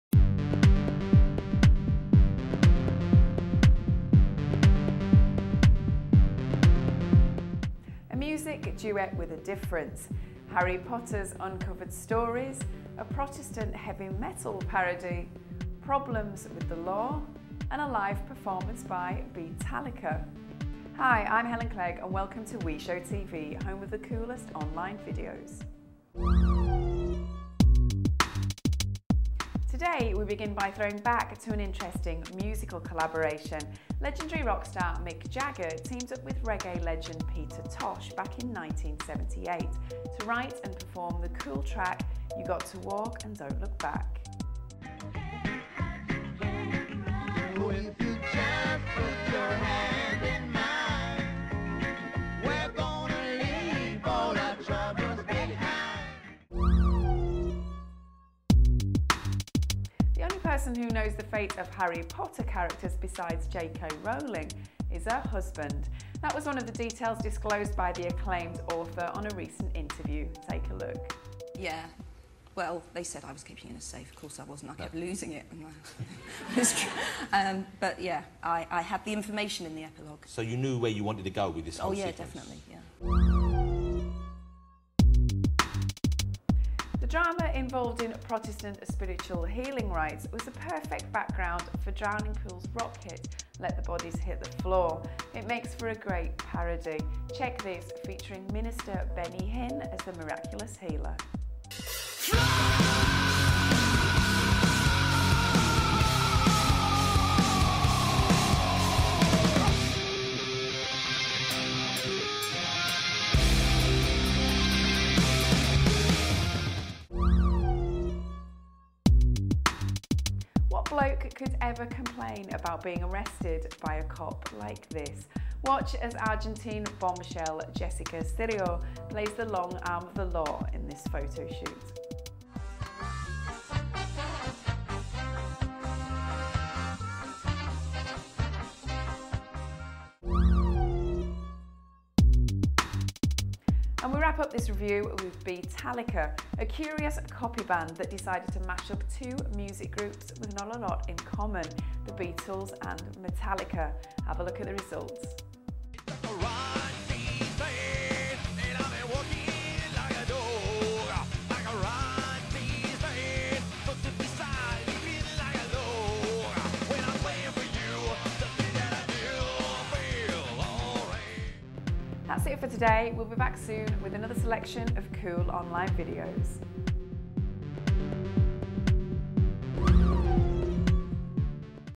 WeShow TV | Daily Review | July 27th
In this edition, you'll see a duet featuring two music legends, Harry Potter's uncovered stories and a protestant heavy metal parody. Also, a sexy photoshooting with a hot female cop and an unusual mix between the Beatles and Metallica. WeShow TV brings you the best in videos everyday.